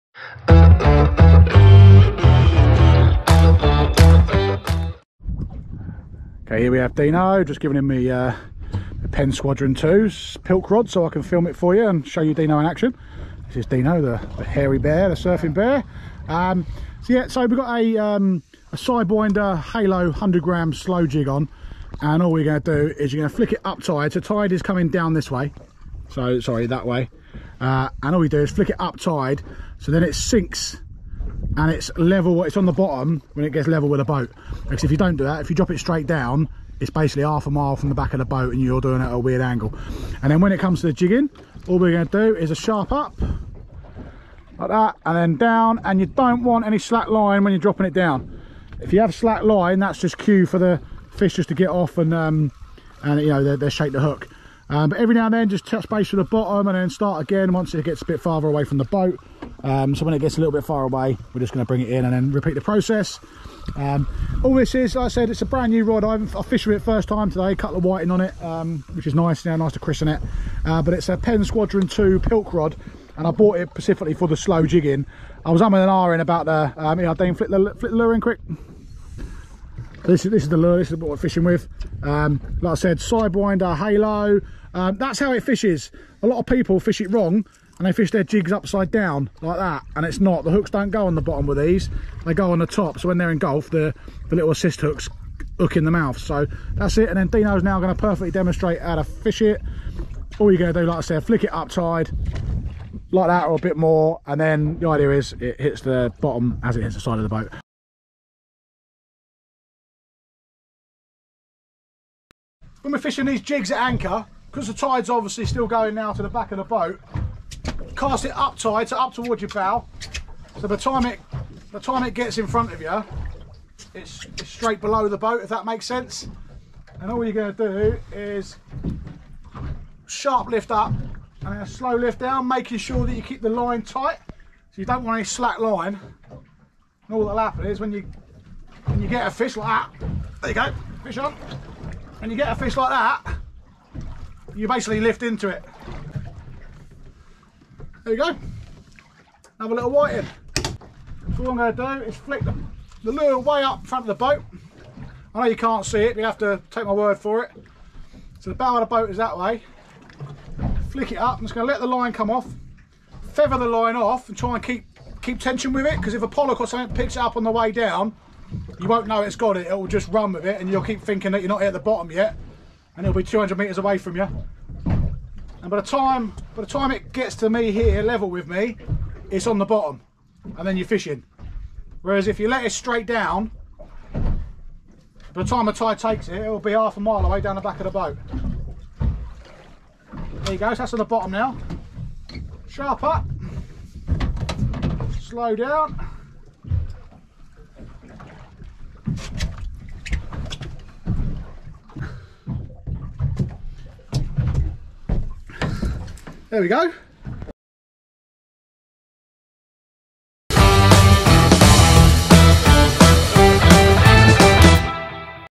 How To Fish A Slow Pitch Jig (SPJ) At Anchor The Basics & Technique To Help Boat A Few More Fish
Back to basics but useful to some as we make the most of our fishing time last trip out and fished a few slow pitch jigs over the slack water and small tide to boat a few extra Whiting and Gurnard while we are at anchor.

Jigging in it’s own right is a great way to catch wreck species on the drift but can also be done while at anchor and is a great way to pass time while waiting for those bites to come on the big bottom baits.

Ben

The Amputee Angler